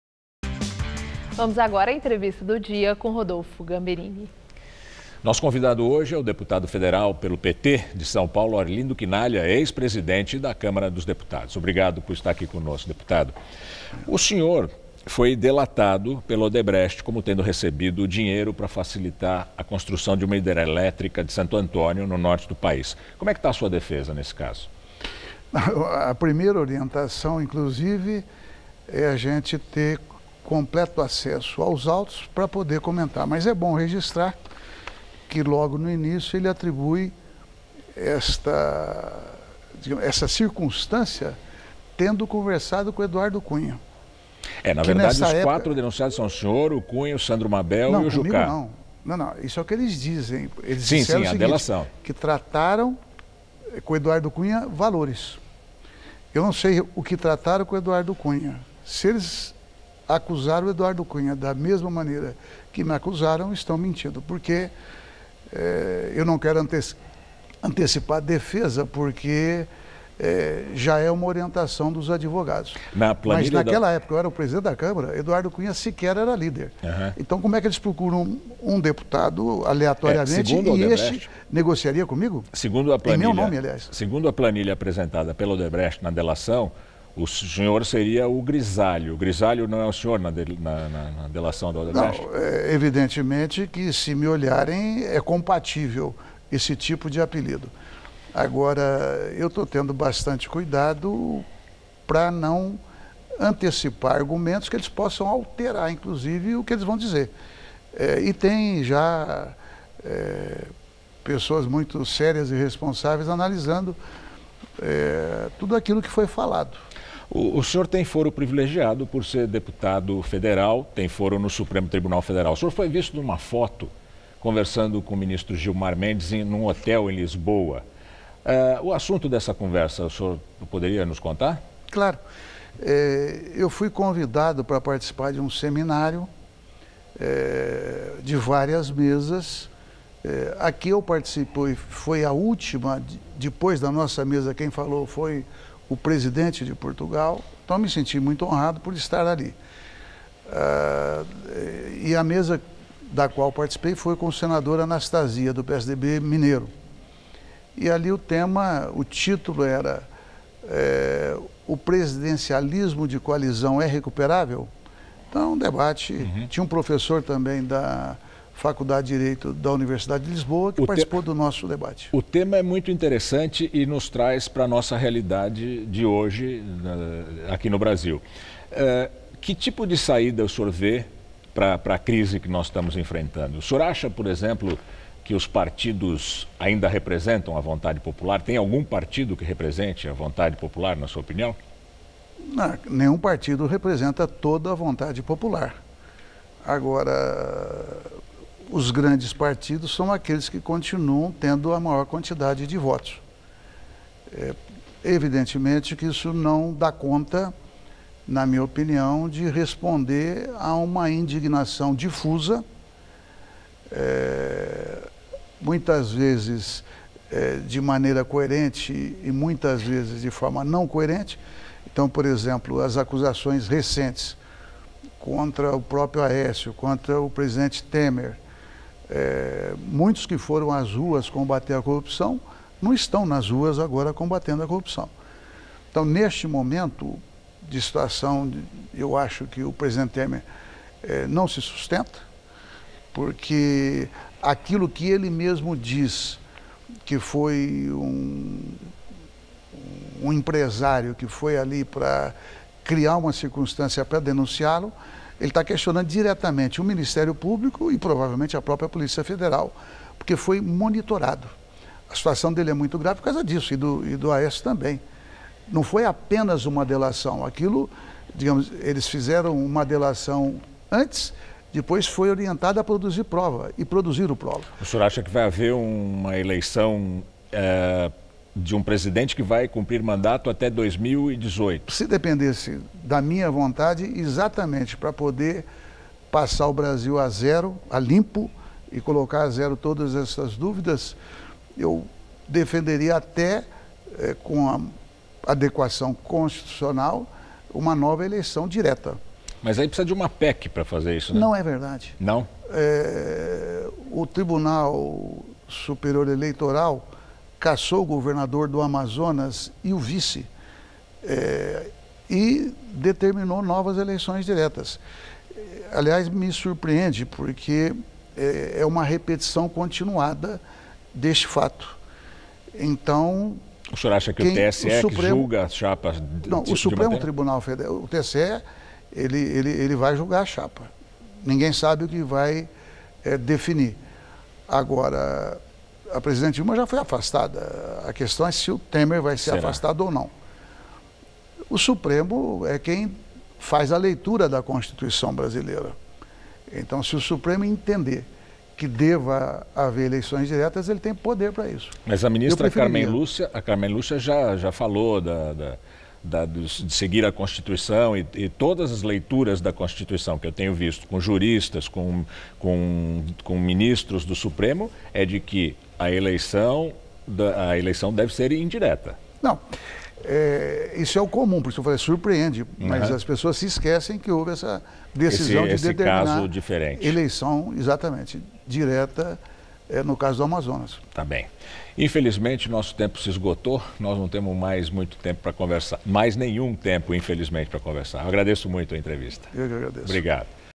Rodolpho Gamberini entrevista Arlindo Chinaglia, dep. federal do PT/SP
Vamos agora à entrevista do dia, com Rodolpho Gamberini.